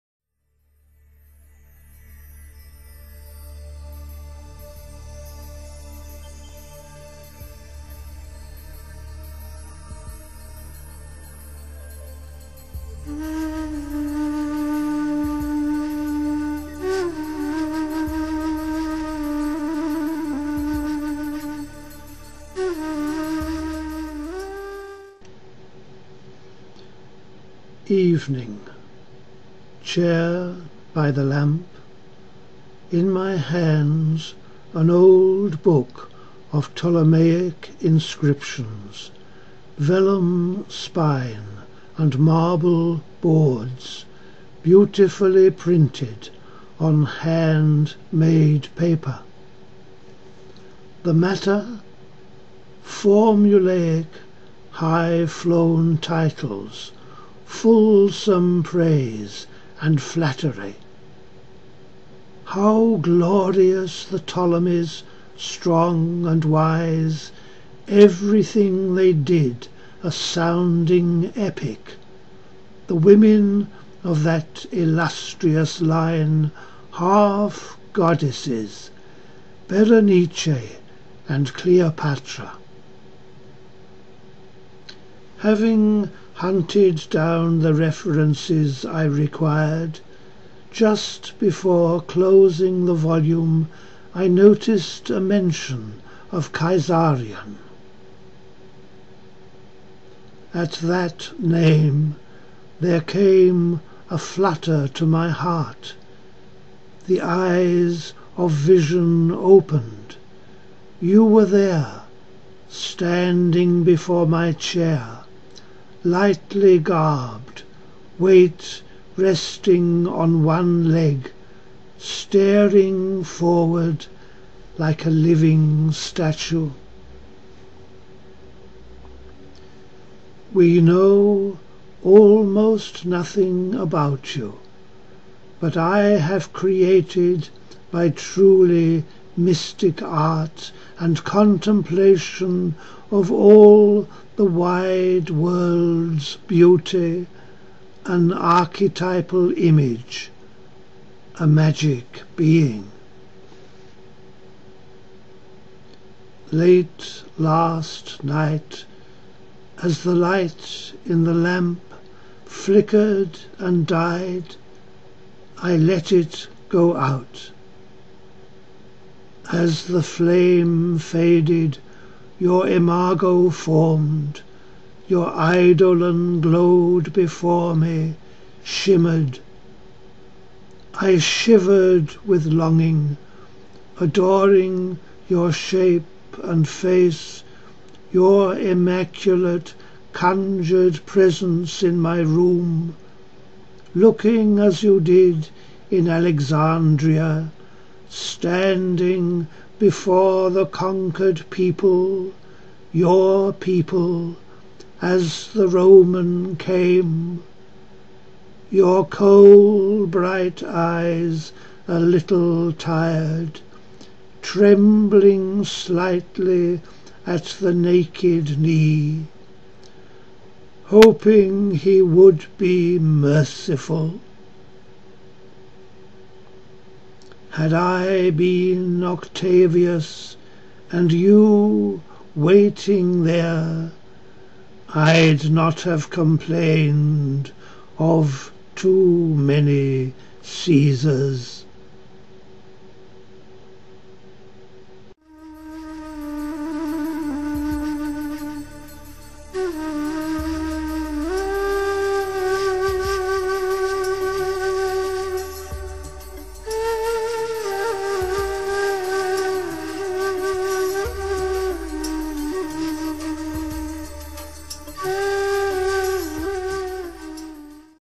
Kaisarion: by Constantine Cavafy
'Kaisarion' is poem no. 73 in the Cavafy canon.

Pronunciation, like spelling, is the very devil. People speaking the same language sometimes fail to understand one another. When ancient Greek and Latin meet English, chaos ensues. The Romans said not 'Hail Zizar!' as in the English version, but 'Ar-way Kizer' (Ave Caesar). The Germans got it right with their Kaiser. The son of Julius Caesar and Cleopatra was called not 'Size-arion' (Caesarion) but Kaisarion.

By August, 30BC, Julius Caesar, Cleopatra and Mark Antony were all dead and Kaisarion was the nominal ruler of Egypt. The invading Octavius Caesar, later called Augustus by the respectful Romans, who was a 'Caesar' because he had been adopted by Julius, coming into the presence of the boy pharaoh Kaisarion at Alexandria, is said to have spoken the words (referring to himself and the boy) "Two Caesars is one too many!" Kaisarion was murdered.

So we drift back (drift back from the present moment in the year 2008AD) to that same city of Alexandria around the year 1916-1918AD and to Cavafy's flat in the Rue Lepsius where, book on knee, he is himself drifting fondly back to the year 30BC and recalling the to him magic being Kaisarion. Wormholes in the fabric of space and time, through which we travel in the Tardis we call Art and Poetry!

KAISARION

Evening. Chair by the lamp. In my hands
an old book of Ptolomaic inscriptions,
vellum spine and boards,
beautifully printed on handmade paper.
The matter: formulaic highflown titles,
fulsome praise and flattery.
How glorious the Ptolomies, strong and wise,
everything they did a sounding epic!
The women of that illustrious line
half goddesses, Berenice and Cleopatra.

Having hunted down the references I required
just before closing the volume
I noticed a mention of Kaisarion.
At that name there came a flutter to my heart.
The eyes of vision opened. You were there,
standing before my chair, lightly garbed,
weight resting on one leg, staring forward
like a living statue.

We know almost nothing about you
but I have created by truly mystic art
and contemplation of all the wide world's beauty
an archetypal image, a magic being.

Late last night as the light in the lamp
flickered and died, I let it go out.
As the flame faded your imago formed,
your eidolon glowed before me, shimmered.
I shivered with longing, adoring your shape and face,
your immaculate conjured presence in my room
looking as you did in Alexandria
standing before the conquered people,
your people, as the Roman came:
your kohl-bright eyes a little tired,
trembling slightly at the naked knee,
hoping he would be merciful.

Had I been Octavius and you waiting there,
I'd not have complained of too many Caesars.